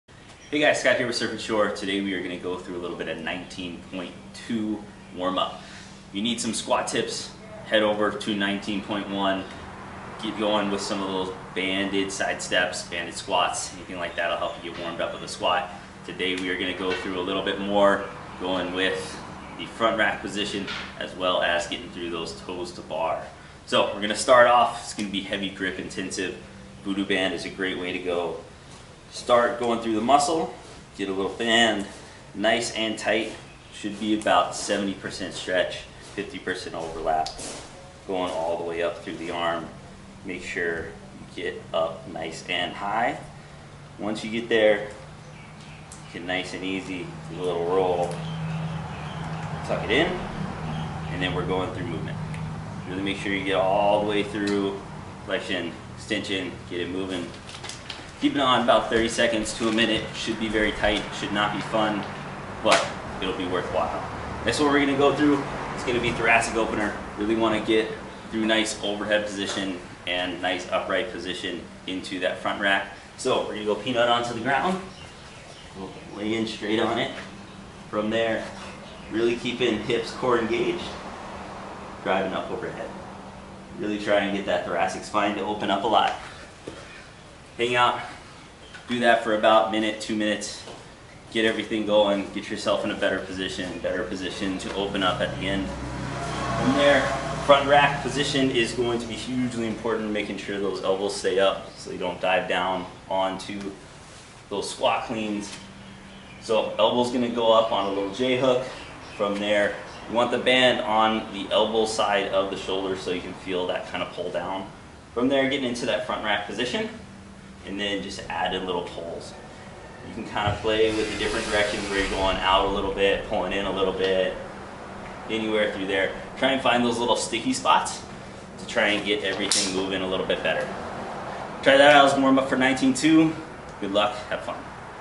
19.2 Prep
Getting after 19.2? Give these a try first to move and perform on your best.